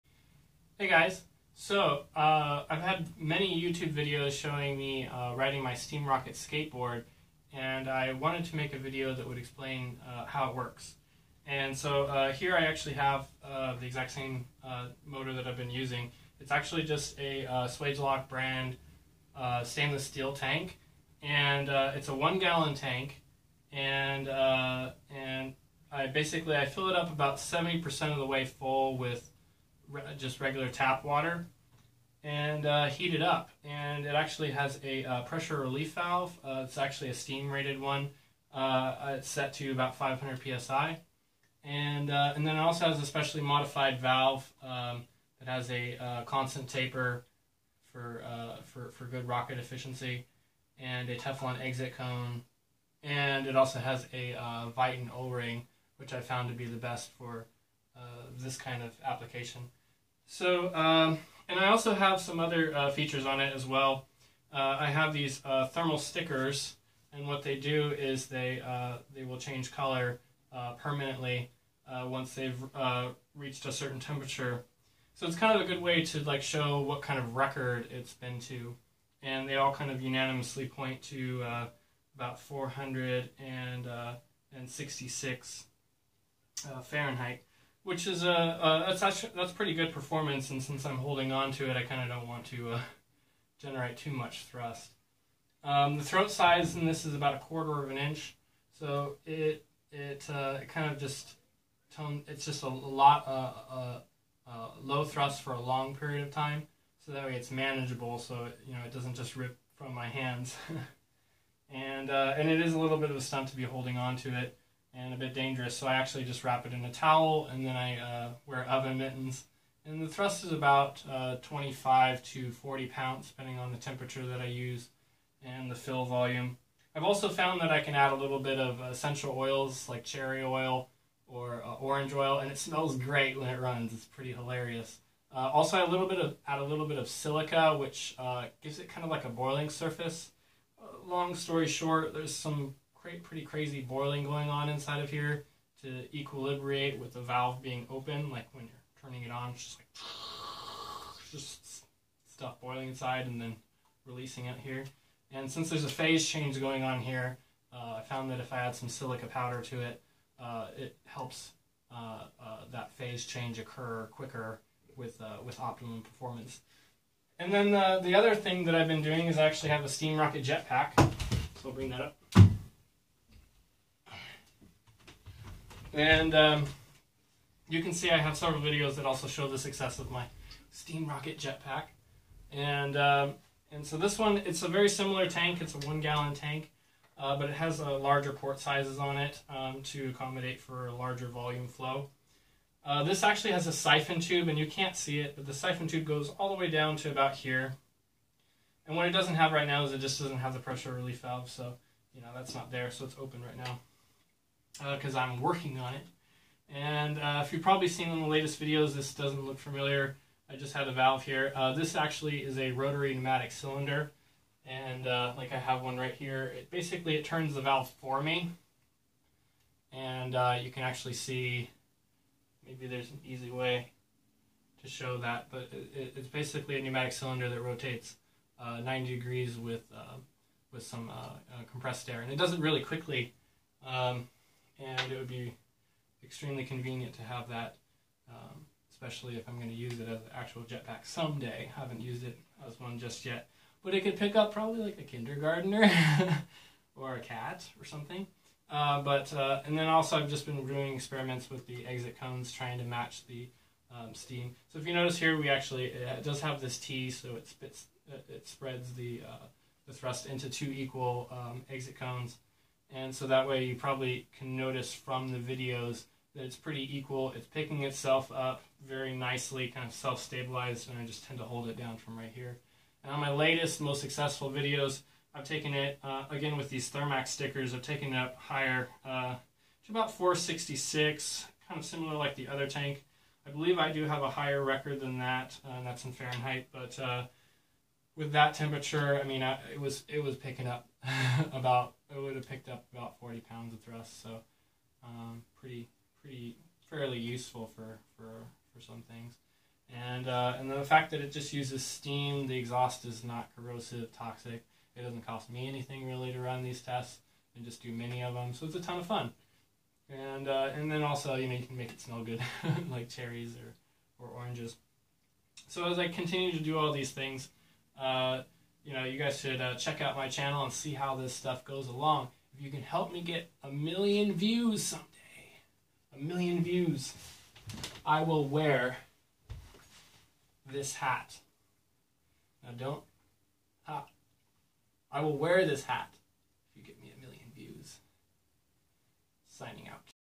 steam rocket explanation! DIY jet pack!
finally I reveal to YouTube how my steam rocket motor and jet pack work!